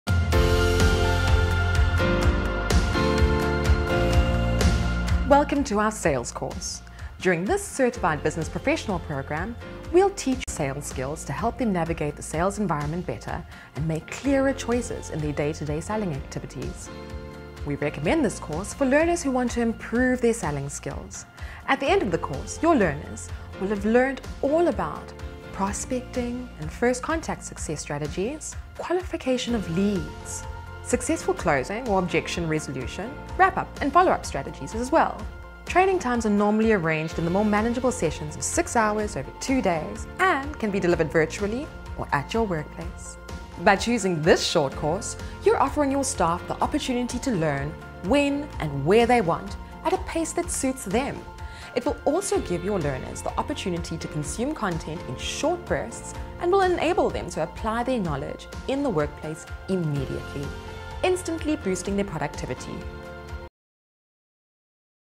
iLearn Sales Short Course
This course lays the foundation for professional selling by developing the selling process using effective sales methodologies. You will learn the skills and the tactics of the leading sales professionals and take part in an interactive scenario to master those skills. The CBP sales module covers all the major sales stages and teaches the best practices in the sales industry.